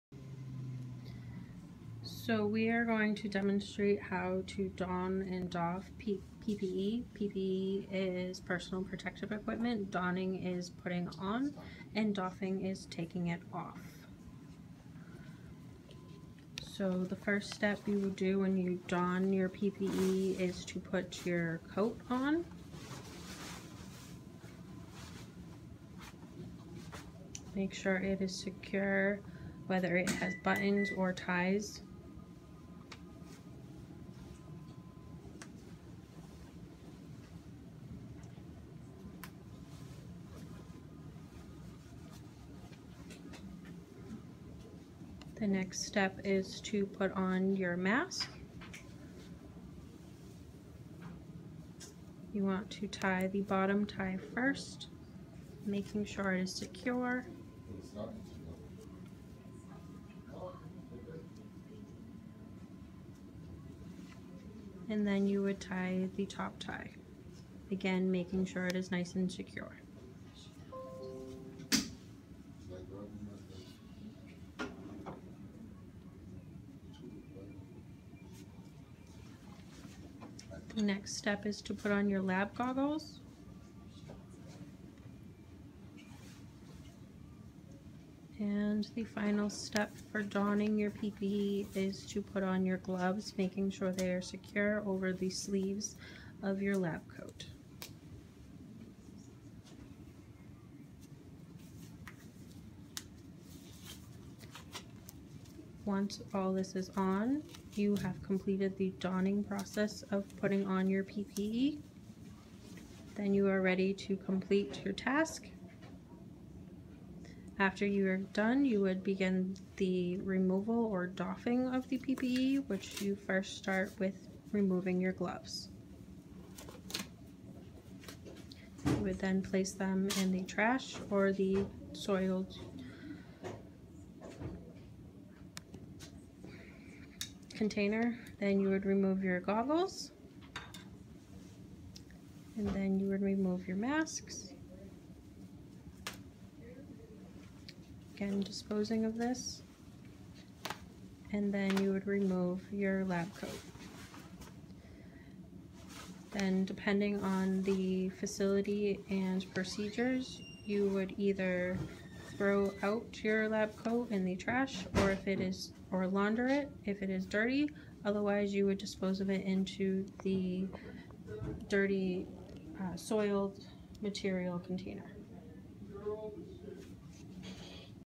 Personal Protective Equipment Use
Student Created video describing the appropriate PPE use.